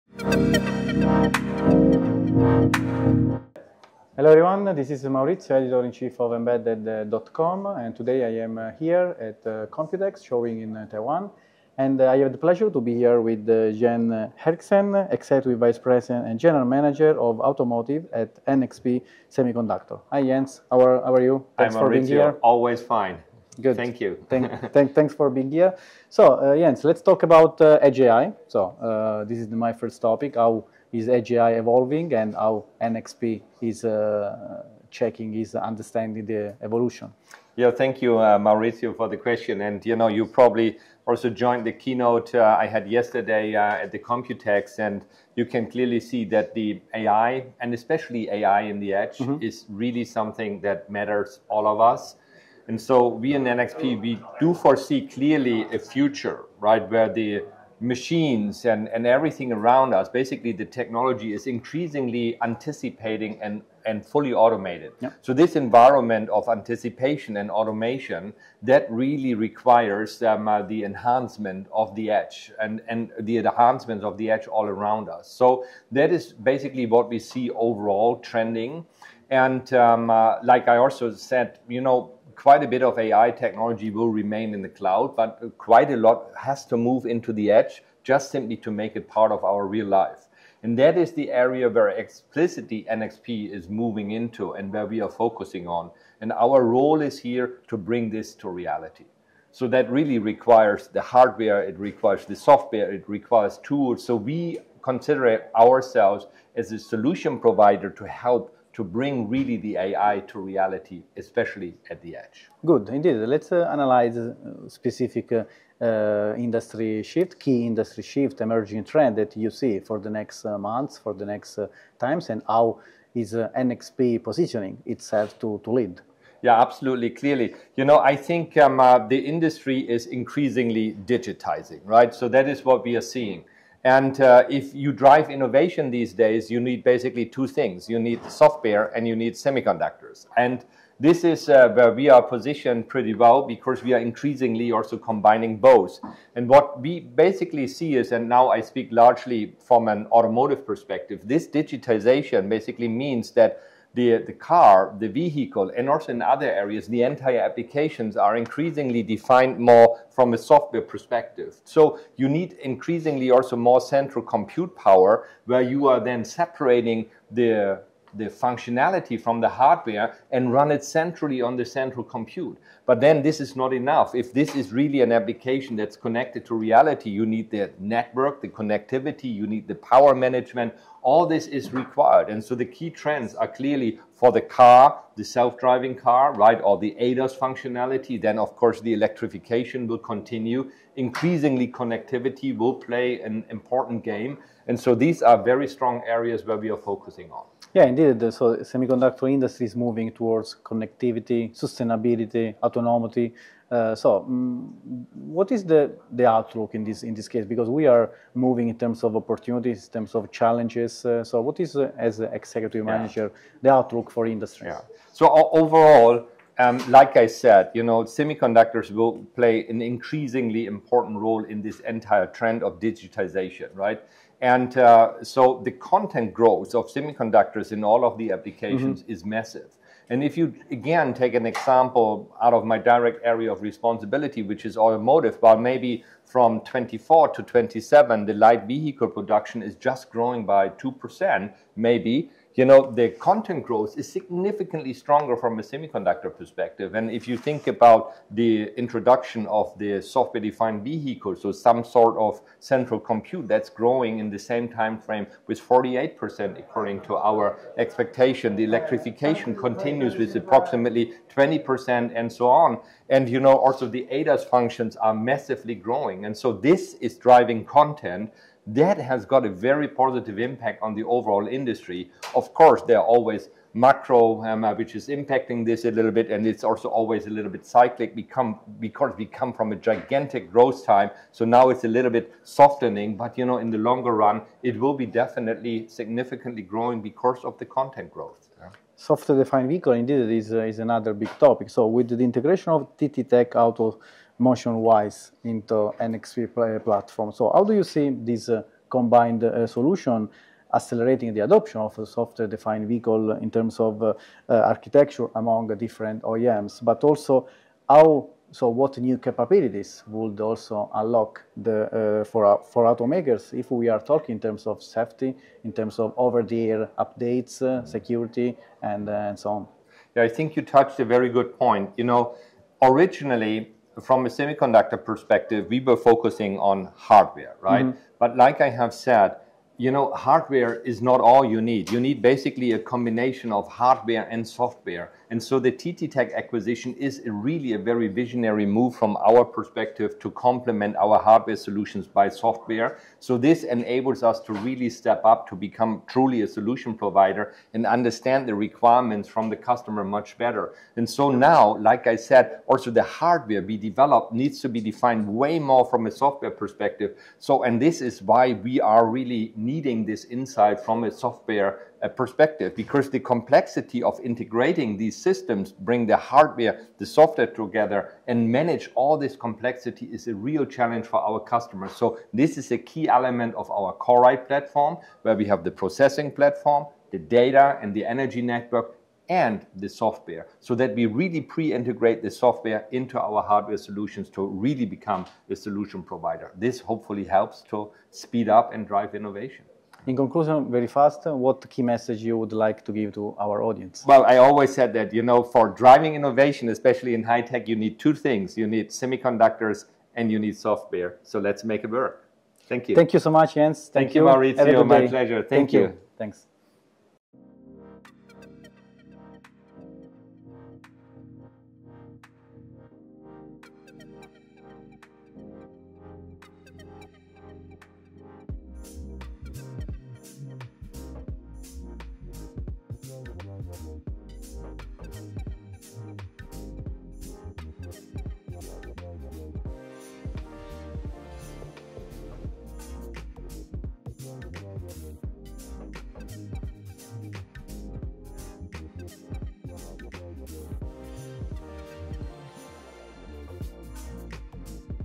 NXP Semiconductors: Leveraging the Edge AI Evolution
NXP：聚焦汽車邊緣AI與SDV架構整合

在2025年台北國際電腦展(Computex 2025)期間，《EE Times》與《Embedded.com》主編Maurizio Di Paolo Emilio專訪恩智浦半導體(NXP Semiconductors)執行副總裁暨類比與車用嵌入式系統事業部總經理Jens Hinrichsen，展開一場深入談論車用AI、軟體定義車(SDV)與未來半導體發展的精彩對話。
「我們看到AI正在從雲端走向邊緣，」Hinrichsen表示，「這樣的轉變對於實現日常生活中的真正智慧化、自動化應用至關重要。」

帶你搶先掌握全球科技新趨勢！

Computex 2025 – NXP Semiconductors: Leveraging the Edge AI Evolution

Jens Hinrichsen, Executive Vice President and General Manager, Analog and Automotive Embedded System, at NXP Semiconductors spoke with Maurizio Di Paolo Emilio, editor-in-chief of Power Electronics News and embedded.com, at COMPUTEX 2025 to talk about how the company is leveraging the evolution happening at Edge AI.